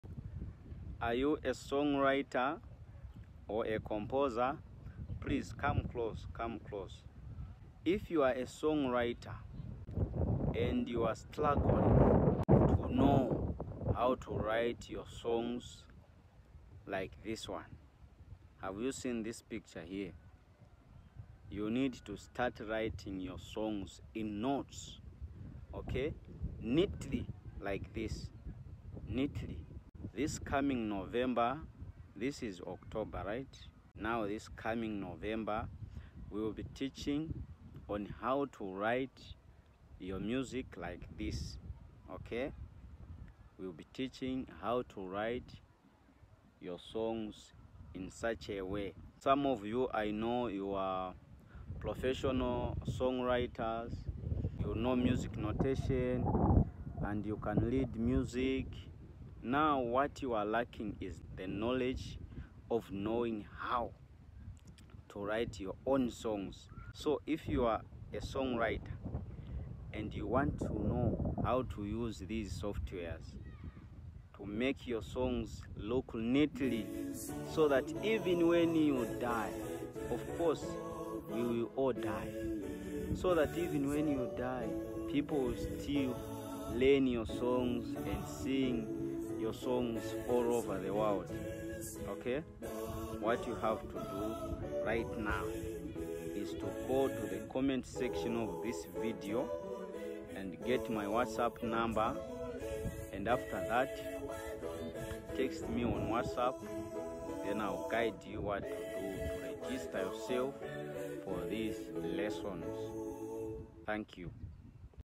How To Write Your Songs In Notes
Learn how to write your own music scores in just few clicks